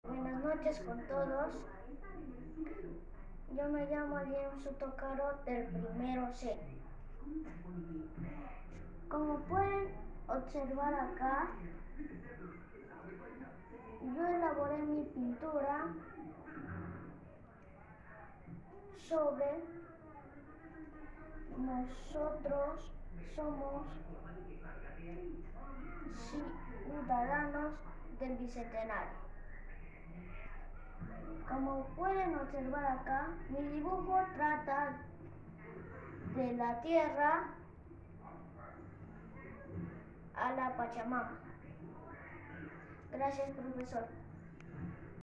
PINTURA - CUIDEMOS NUESTRA TIERRA
La pintura está inspirada en el lema “Nosotros Somos los Ciudadanos del Bicentenario” se elaboró con temperas, y trata de que la tierra o “Mama Pacha” pide que la cuidemos y no la contaminemos ya que nuestro planeta esta sufriendo con mucho dolor.